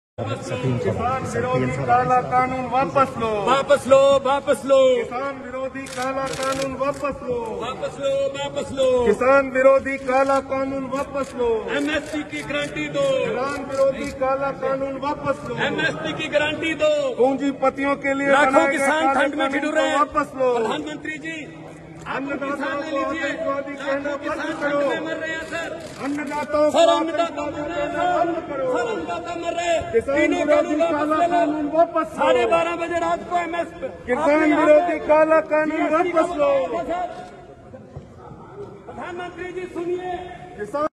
PM ਨਰਿੰਦਰ Modi, ਦਾ ਕੀਤਾ ਵਿਰੋਧ Bhagwant mann
ਭਗਵੰਤ ਮਾਨ ਨੇ ਕੀਤੀ ਨਾਅਰੇਬਾਜ਼ੀ ਕਿਹਾ ਕਾਲੇ ਕਾਨੂੰਨ ਵਾਪਸ ਲਓ , ਨਰਿੰਦਰ ਮੋਦੀ ਨੂੰ ਪਈ ਹੱਥਾਂ ਪੈਰਾਂ ਦੀ